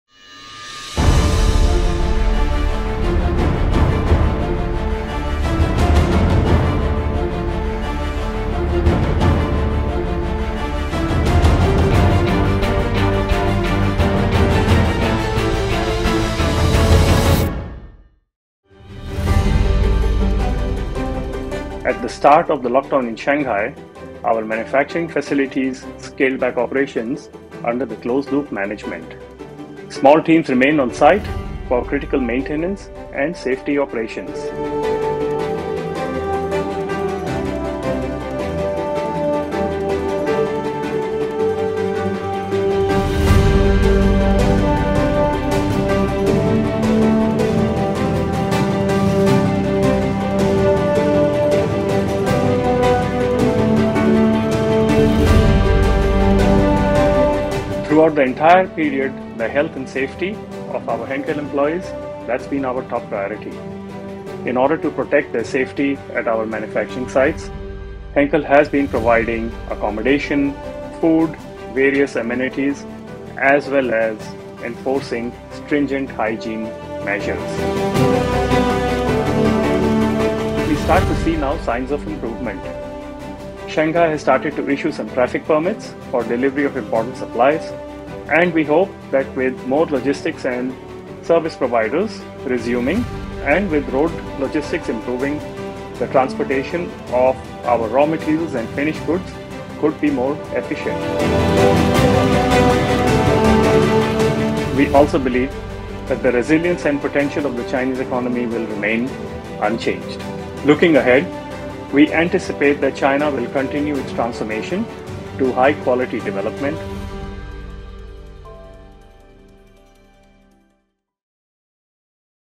Henkel resumes operation and gives confidence in Shanghai
As of May 8, the production of Henkel’s plants is getting back on track. Dr. Rajat Agarwal, President of Henkel Greater China, said operation resumption is a very positive sign, which gives confidence to their customers and employees.